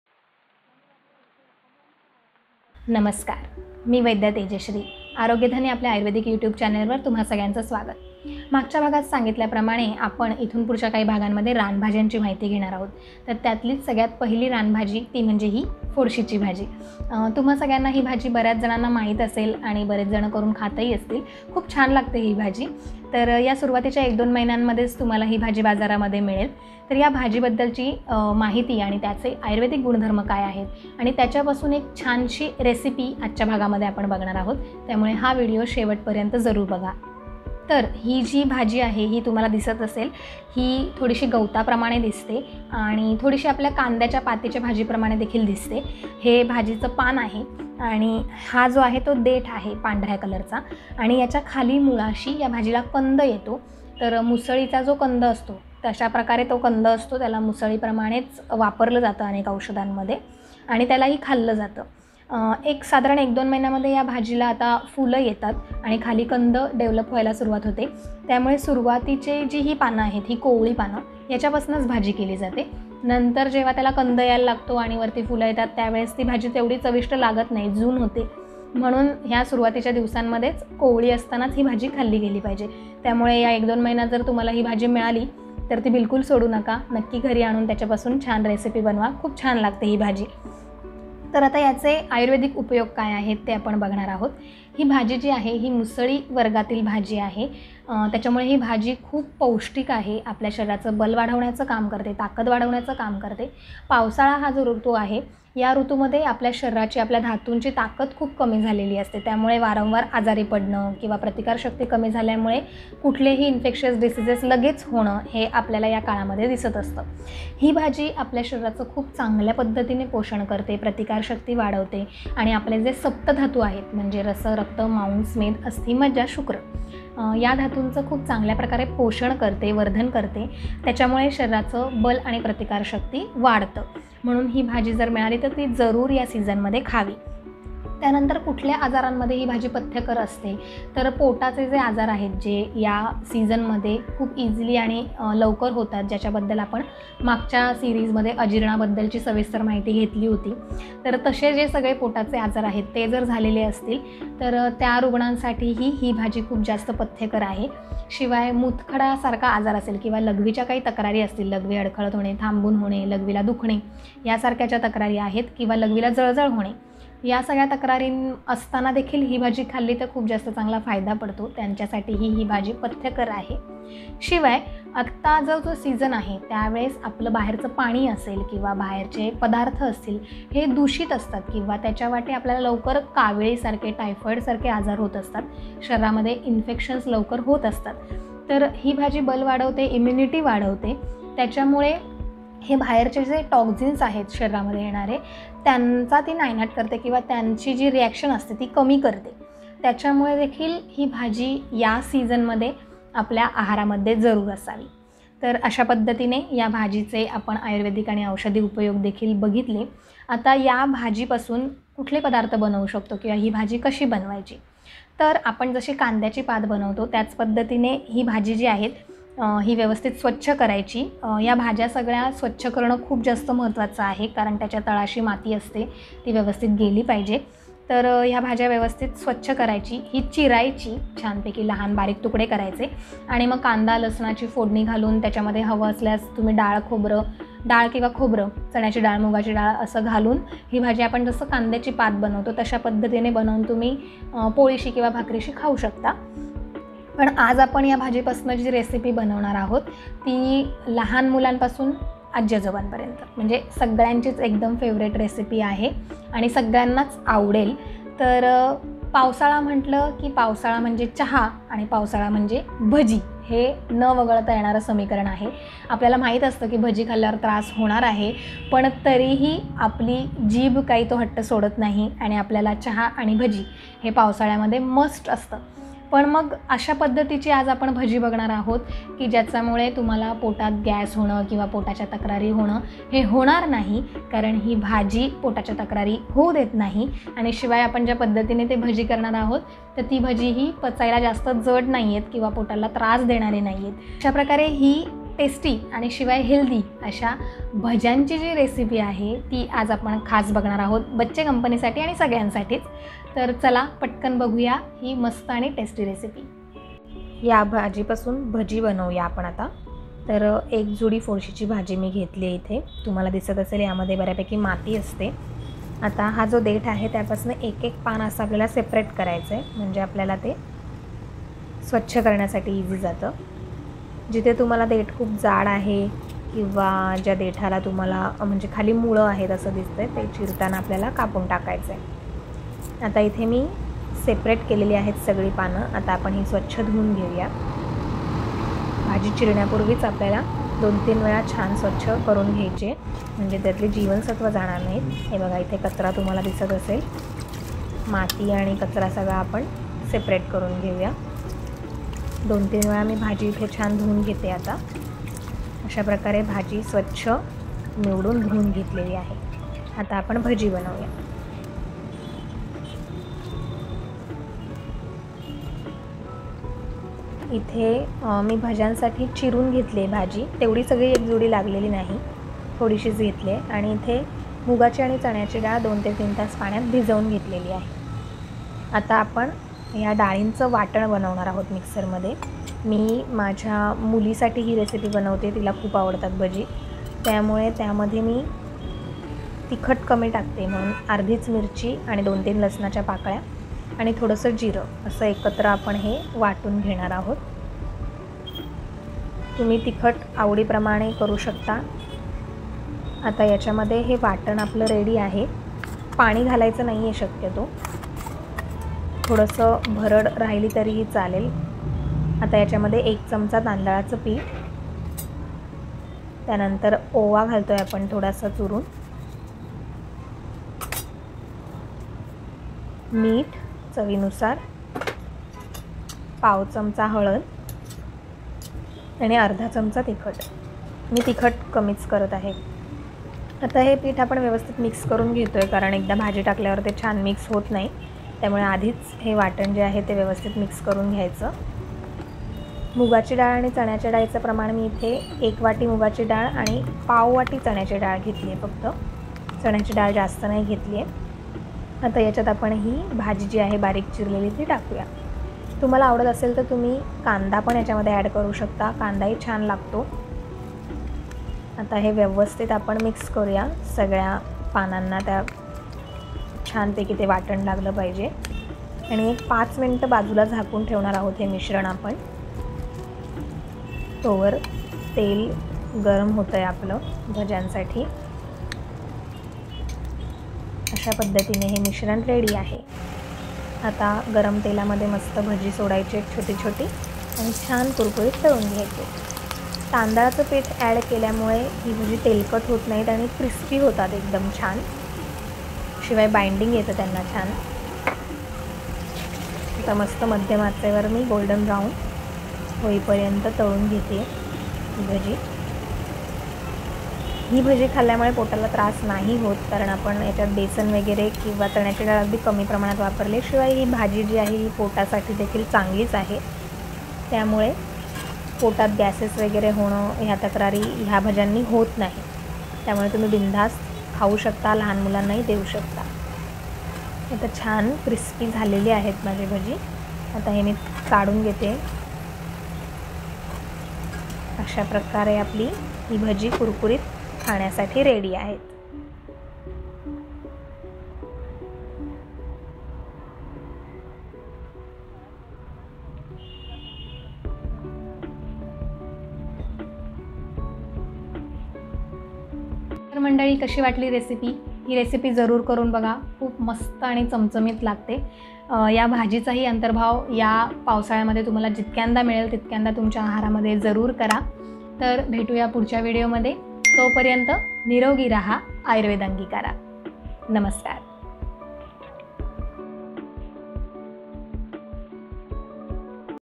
फोडशी/ कुलू ची भजी / फोडशीच्या भाजीचे फायदे/ phodshi bhaji recipe and benefits/लहान मुलांसाठी रेसिपी
This is monsoon special wild vegetable.

 scientific name - Chlorophytum Borivilianum

Other names - Safed Mulsi, Mushli, Karli, Kuli, Peva

It is a very nutritious vegetable. It helps to improve immunity of body . According to Ayurveda it is Balvardhak i.e. increases strength of all dhatus. Also it is advisable to have for patients having digestive problems , urinary problems , lack of immunity.
  Here we are giving one tasty and crispy recipe of this vegetable i.e. crispy pakoda . This is must have recipe in monsoon . Do try these pakodas and enjoy rainy season.